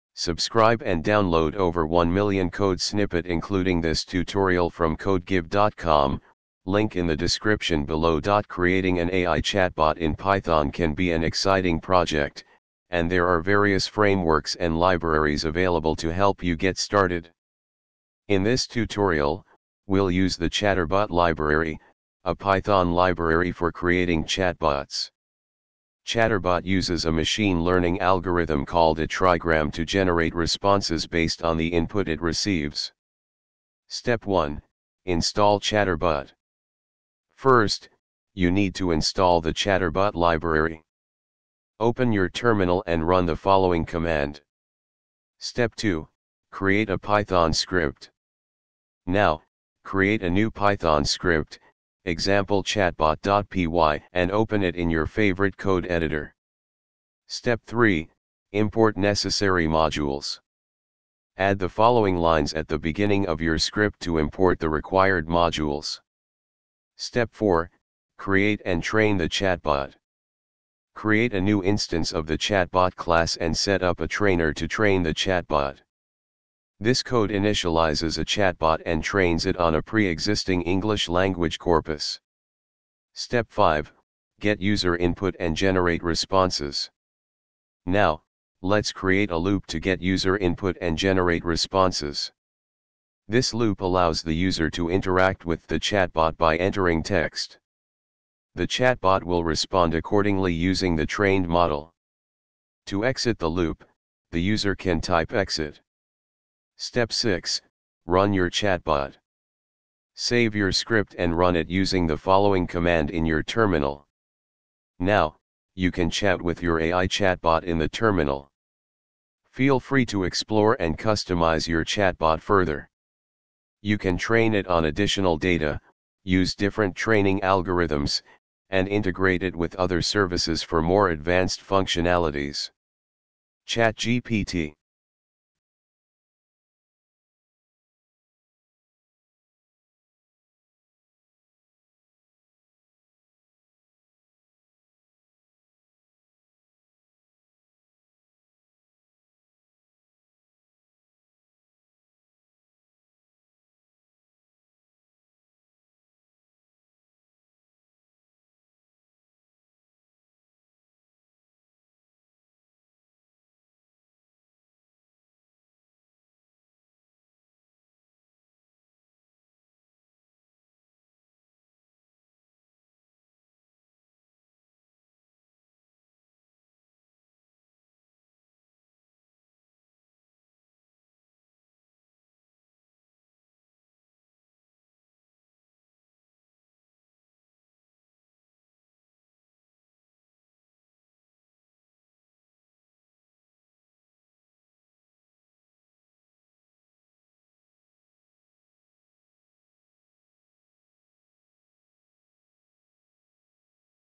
how to create an ai chatbot in python
Creating an AI chatbot in Python can be an exciting project, and there are various frameworks and libraries available to help you get started. In this tutorial, we'll use the ChatterBot library, a Python library for creating chatbots. ChatterBot uses a machine learning algorithm called a "trigram" to generate responses based on the input it receives.
Step 1: Install ChatterBot
First, you need to install the ChatterBot library. Open your terminal and run the following command:
Step 2: Create a Python script
Now, create a new Python script (e.g., chatbot.py) and open it in your favorite code editor.
Step 3: Import necessary modules
Add the following lines at the beginning of your script to import the required modules:
Step 4: Create and train the chatbot
Create a new instance of the ChatBot class and set up a trainer to train the chatbot:
This code initializes a chatbot and trains it on a pre-existing English language corpus.
Step 5: Get user input and generate responses
Now, let's create a loop to get user input and generate responses:
This loop allows the user to interact with the chatbot by entering text. The chatbot will respond accordingly using the trained model. To exit the loop, the user can type 'exit.'
Step 6: Run your chatbot
Save your script and run it using the following command in your terminal:
Now, you can chat with your AI chatbot in the terminal.
Feel free to explore and customize your chatbot further. You can train it on additional data, use different training algorithms, and integrate it with other services for more advanced functionalities.
ChatGPT